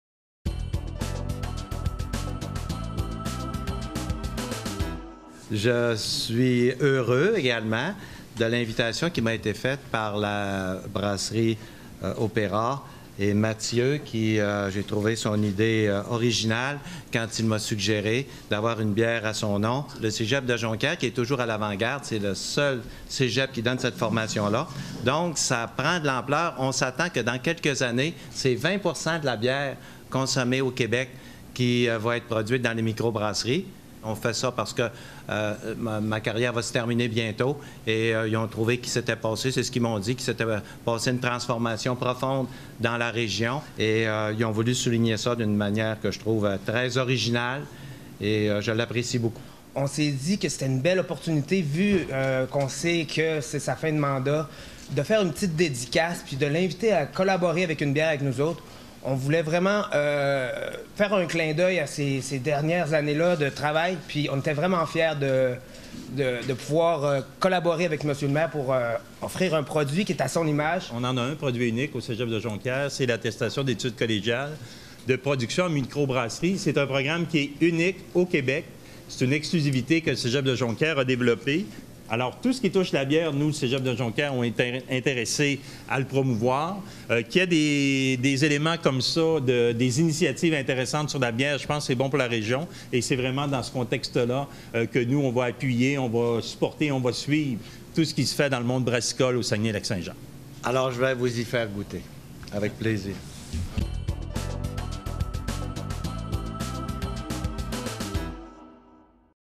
Saguenay, maître en microbrasserie
Le lancement de la bière « Pour-toi-Jean» a eu lieu ce matin en collaboration avec la microbrasserie HopEra et le Cégep de Jonquière.